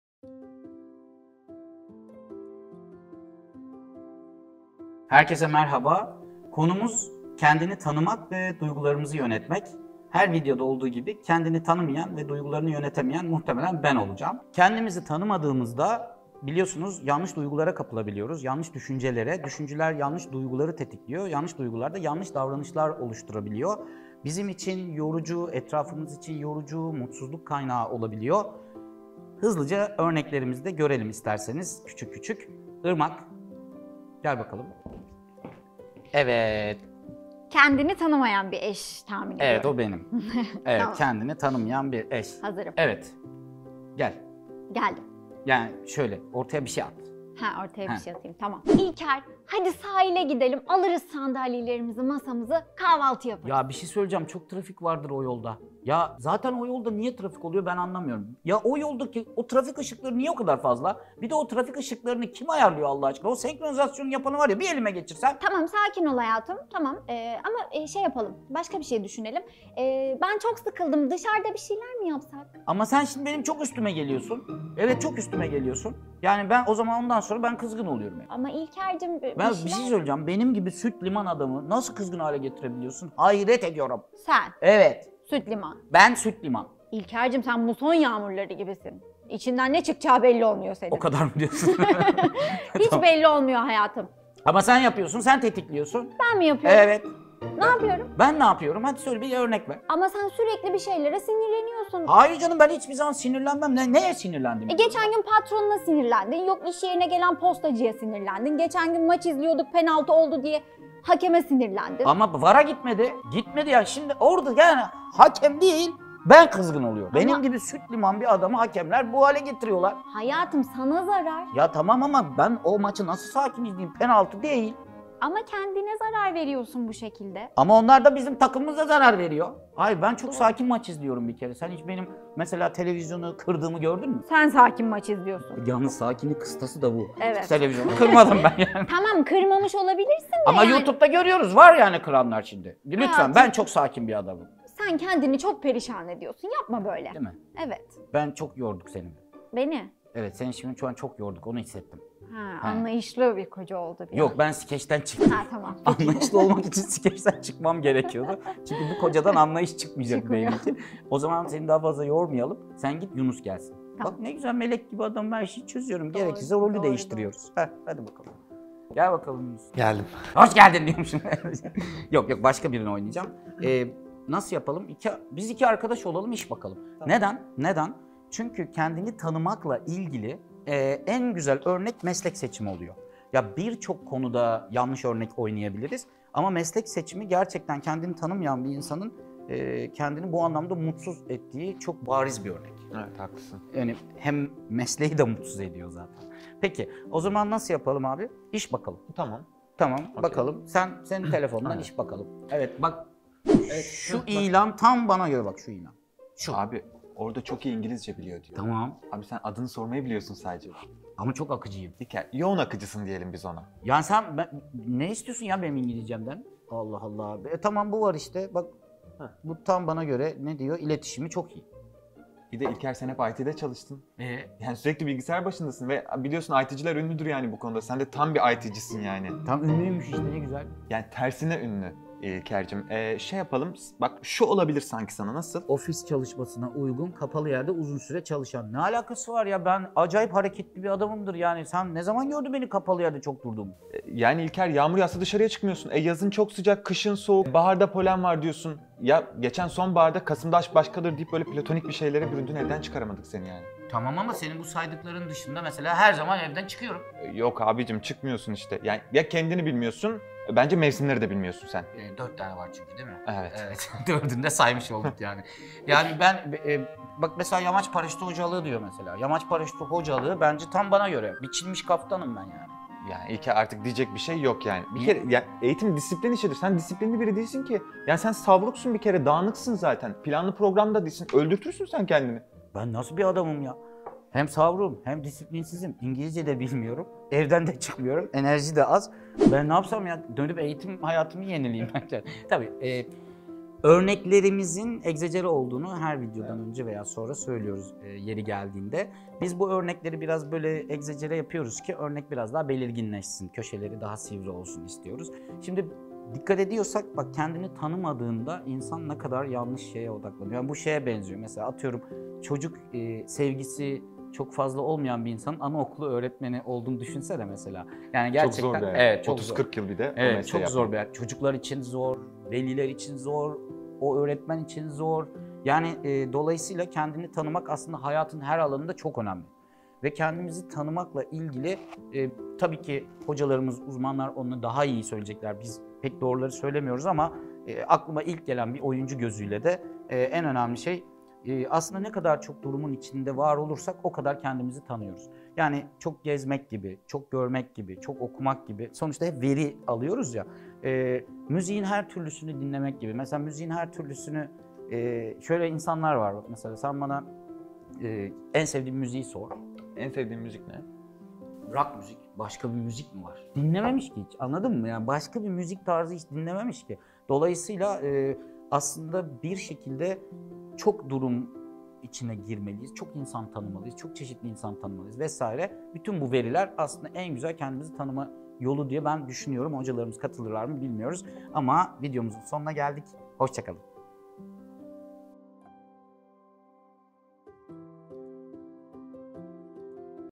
Kendini Tanımak ve Duygularını Yönetmek
Kendimizi tanımadığımız zaman, yanlış düşüncelere kapılabiliyoruz. Yanlış düşünceler, yanlış duyguları tetikliyor; yanlış duygular tetiklendiği zaman yanlış davranışlarda bulunabiliyoruz. Sadece bizim için değil, çevremiz için de yorucu ve mutsuzluk kaynağı durumlar yaşanabiliyor.

Meslek seçiminden, karşılaştığımız gündelik durumlarda yaptığımız tercihlere kadar, yaşamda mutlu olabilmenin ilk koşullarından biri de kendini tanımak. Kendimizi tanıdığımız zaman duygularımızı yönetmek daha kolay olduğu gibi, yanlış davranışlarda bulunmanın da önüne geçecektir.

Eğlence Mühendisleri ekibinden İlker Elibol, Irmak Karakoç ve Yunus Emre Can, hazırladıkları videoda, yaşamdan örneklerle, “Kendini tanımak bize ne katar?”, “Kendini tanımak için ne yapmalıyım?”, “Kendimizi tanımak neden önemli?”, “İnsanın kendini tanıması ne demek?” gibi sorulara yanıt ararken kendini tanımanın önemini örneklerle sergiliyorlar.